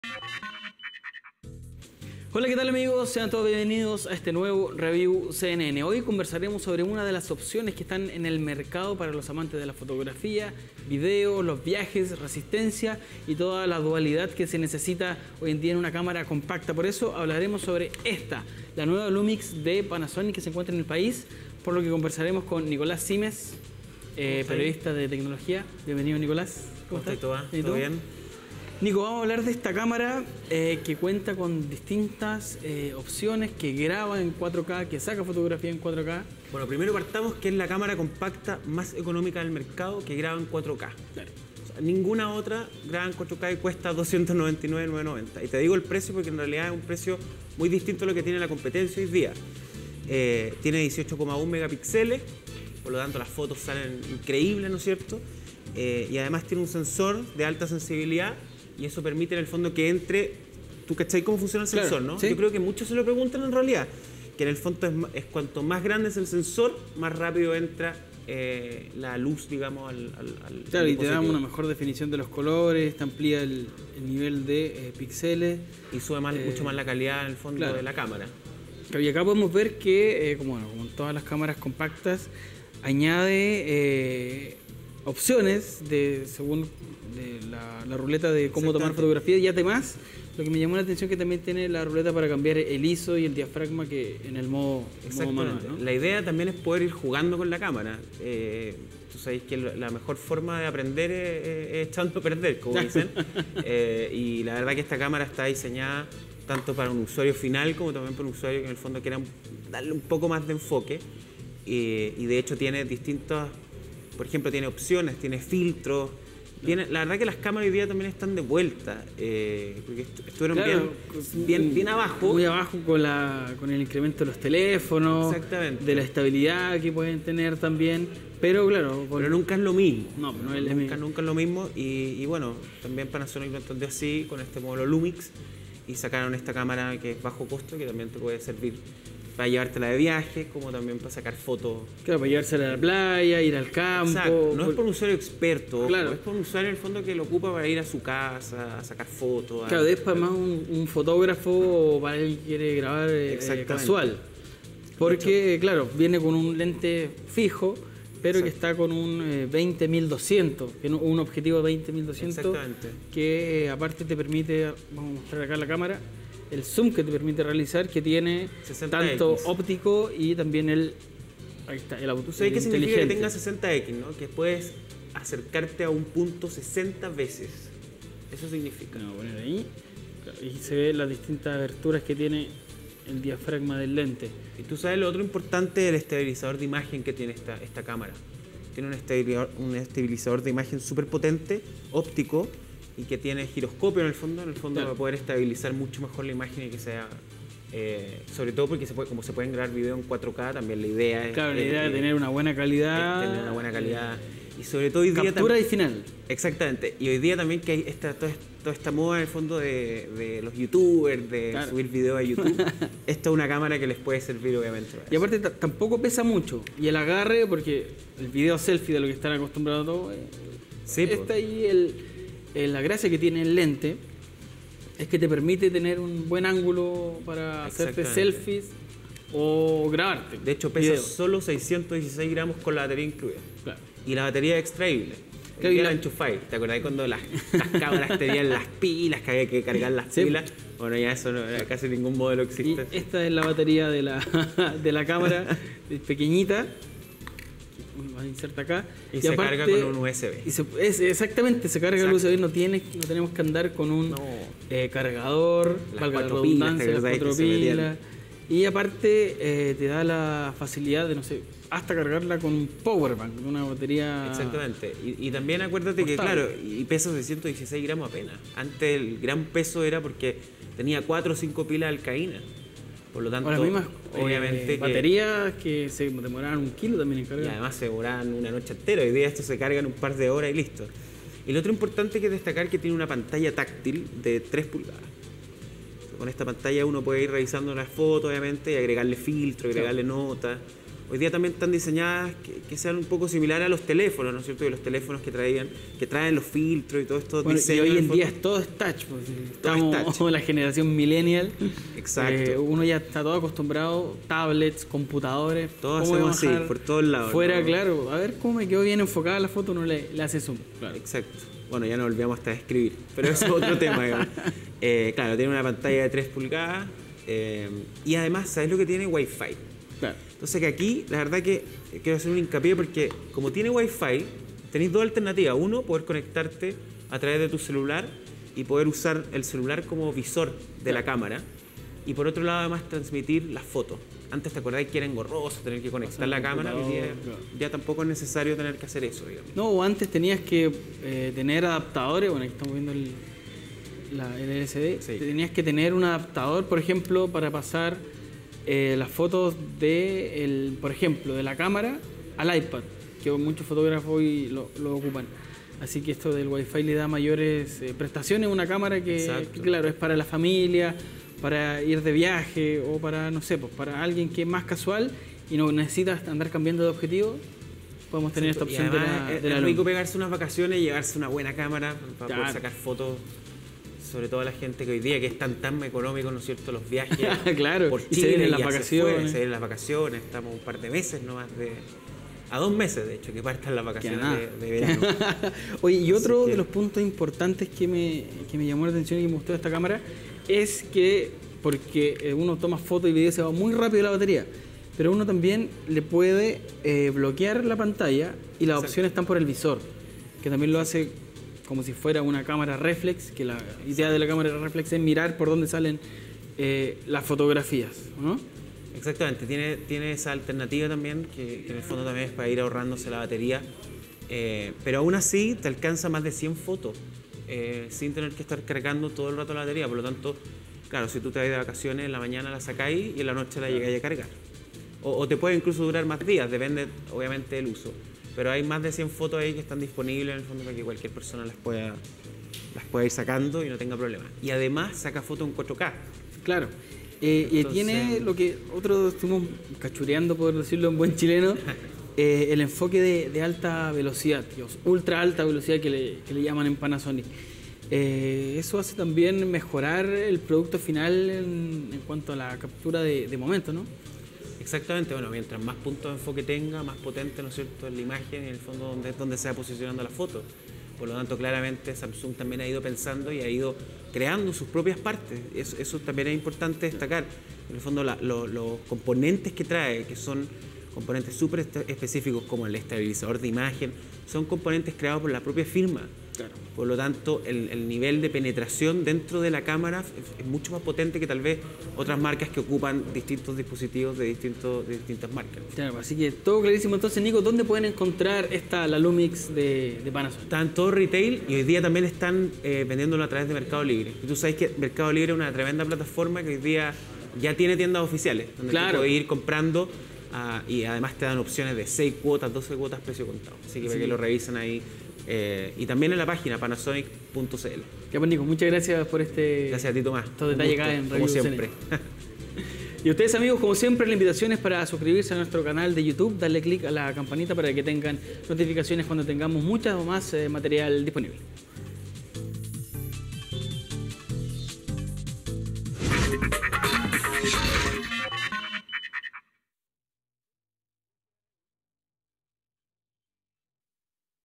Review CNN: La cámara compacta que te da calidad 4K con un lente multifuncional
Revisamos la cámara fotográfica diseñada para quienes no quieren perder ningún recuerdo.